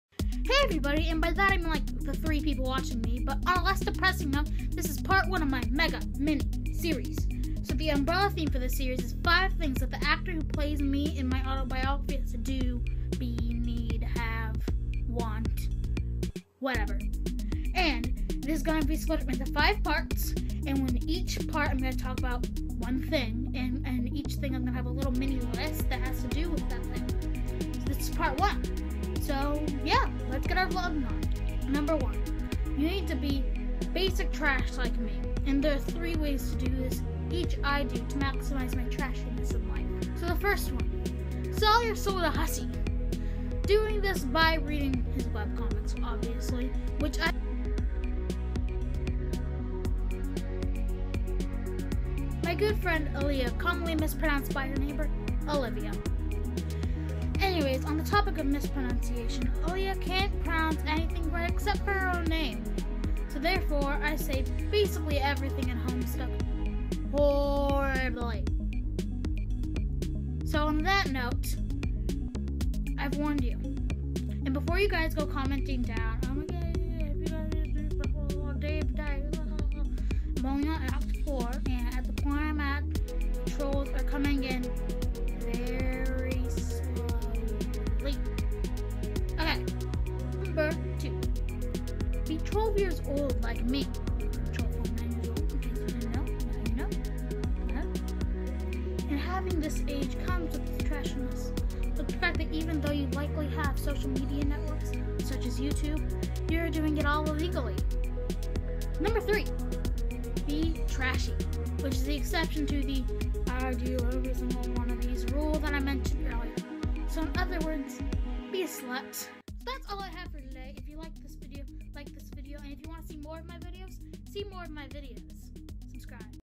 Selling my Soul to Hussie | Pacific Cheese
Today I made a video (YAY) discussing the 3-step process to become to trashy! Hope y'all have a sPACIFICly swell day!